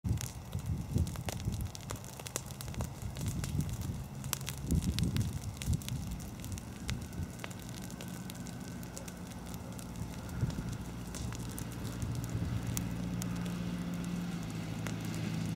Burning Field
Prescribed burning to clean up the ground for Dove.